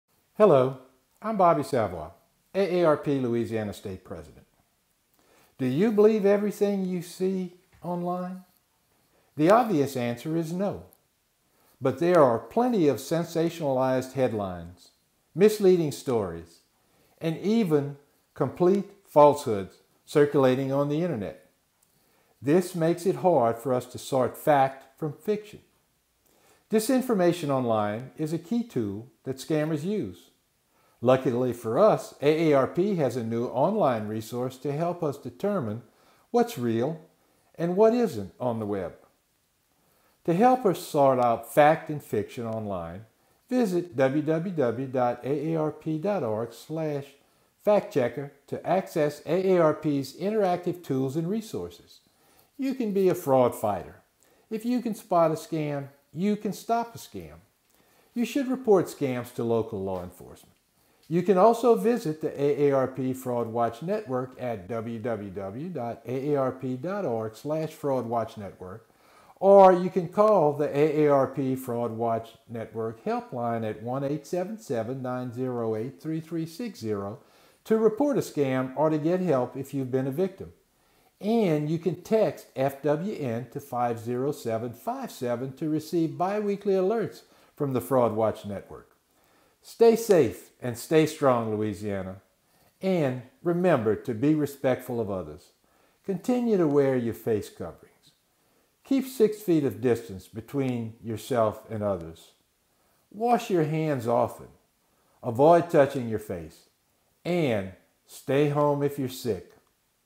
Fraud Watch Friday: FWN Fact Tracker
Bobby Savoie discuss a new fraud tracker to help you sort fact from fiction online.